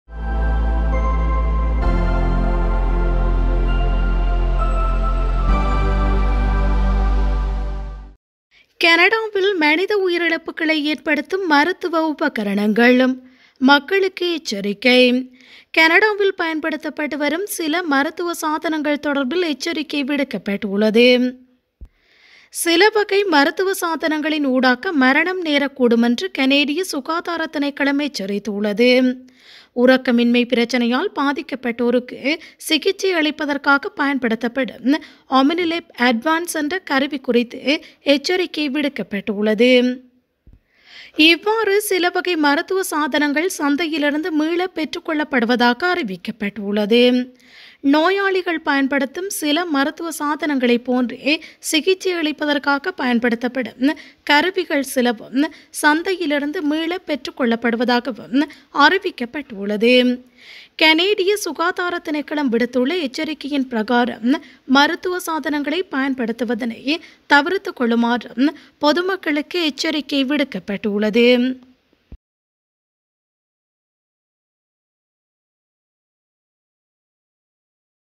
கனடாவில் மருத்துவ உபகரணங்கள் தொடர்பில் மக்களுக்கு எச்சரிக்கை
கனடாவில் பயன்படுத்தப்பட்டு வரும் சில மருத்துவ சாதனங்கள் தொடர்பில் எச்சரிக்கை விடுக்கப்பட்டுள்ளது.